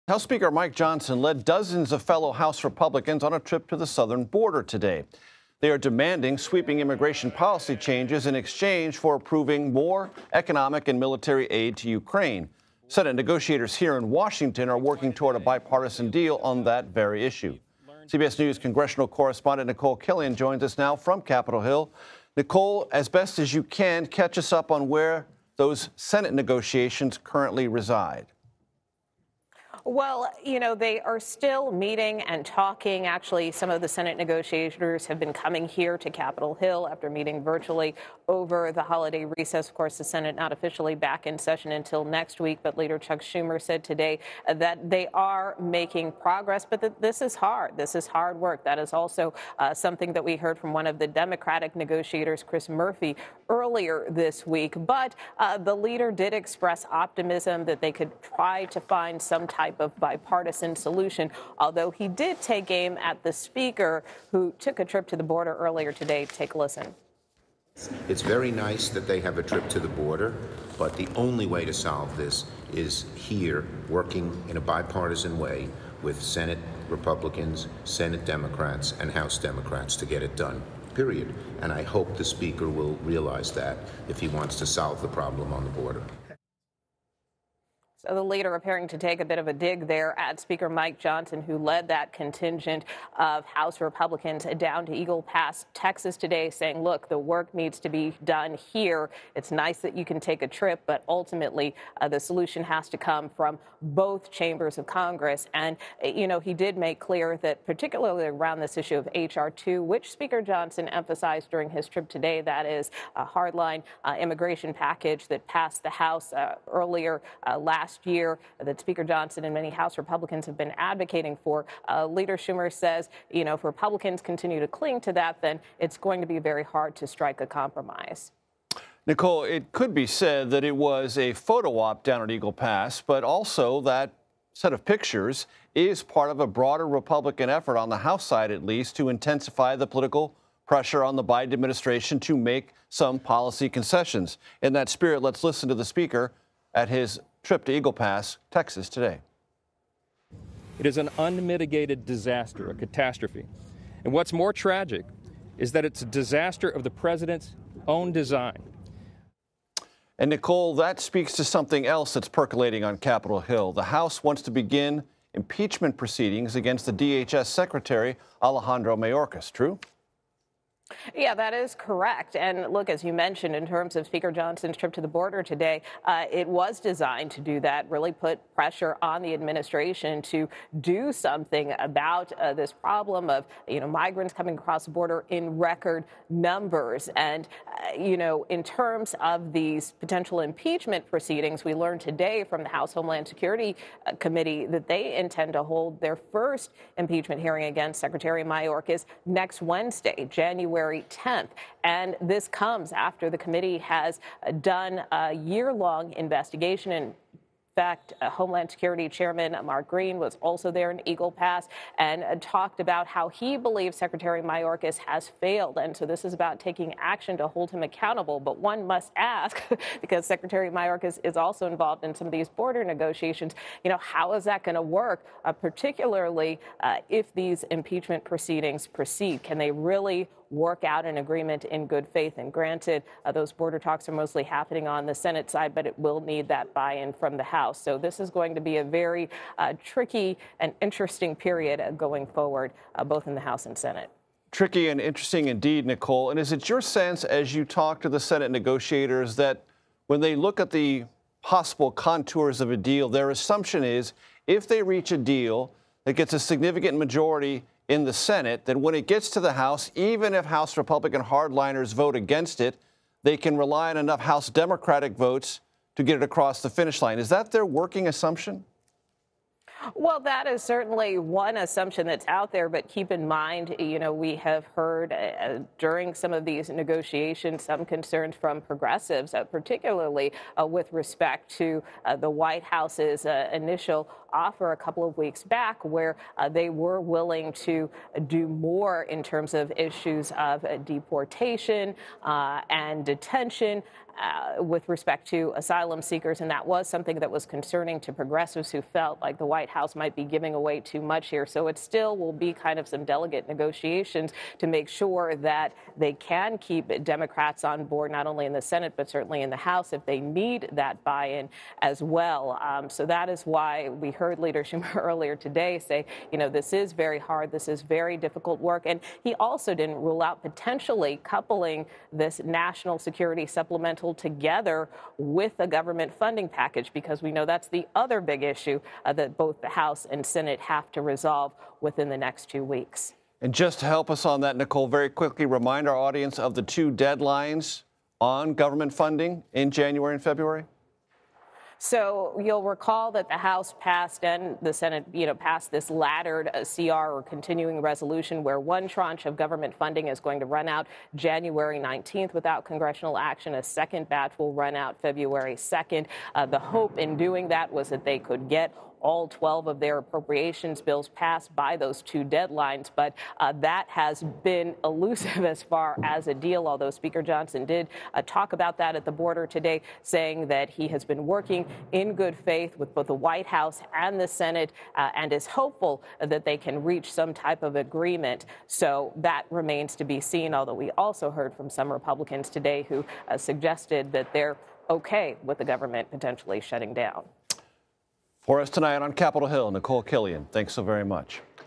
Schumer takes dig at Johnson for visiting border amid deal talks
House Speaker Mike Johnson led dozens of fellow Republican lawmakers on a trip to the southern border Wednesday as Senate negotiators work to strike a deal on border security. CBS News congressional correspondent Nikole Killion reports.